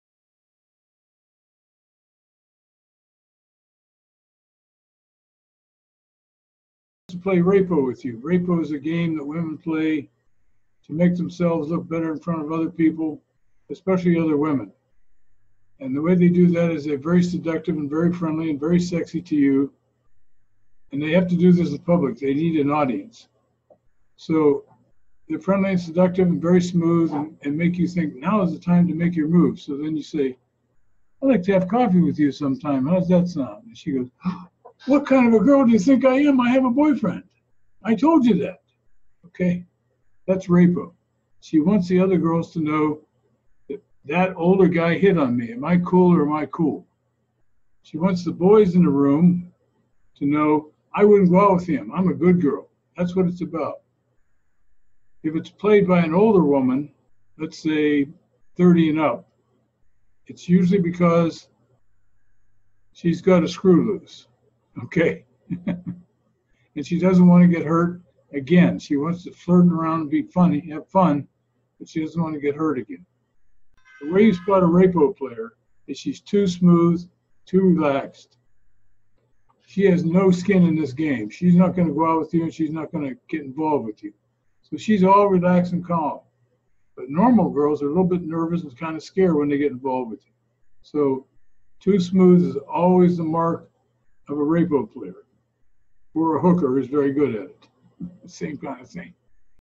RAPO AS IN RAPE O GAME WOMEN PLAY
This is a GAME from Games People Play, Berne circa 1970. played by women is a way of looking good to other women and males they want as a boyfriend. SPOT THEM BY HOW SMOOTH AND REAXED THEY ARE. See updated Date Young Women